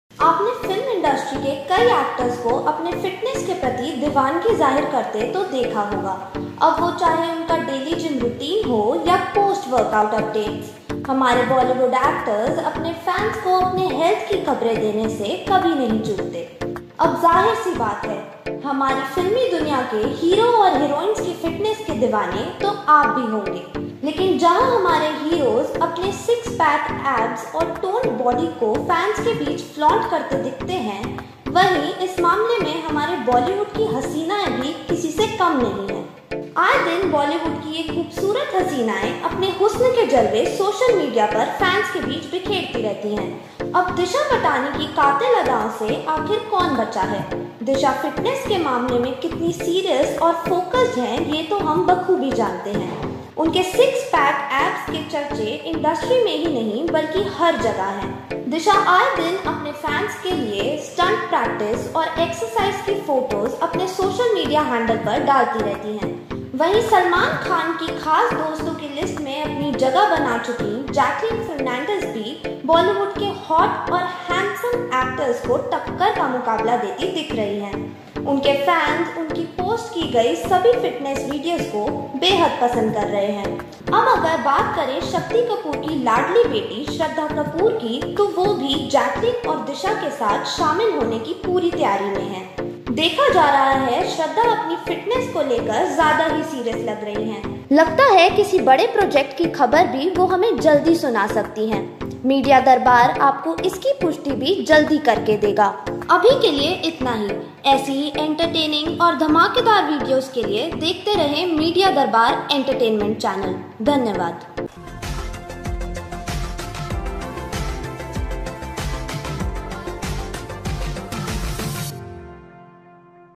Hero से कम नहीं है हमारी ये Bollywood की हेरोइने | Media Darbar
हमारे हीरो अपने सिक्स पैक एब्स और अपनी टोंड बॉडी को अपने फैन्स के बीच फ्लोंट करते दिखते है वही इस मामले में हमारी बॉलीवुड की हसीनाए भी कुछ कम नहीं है | आए दिन बॉलीवुड की ये खूबसूरत हसीनाए अपने हुस्न के जलवे सोशल मीडिया पर फैन्स के बीच बिखेरती रहती है | पूरी खबर देखें लास्ट तक जोकि  ईशिका द्वारा खास आपके लिए पेश की गयी है |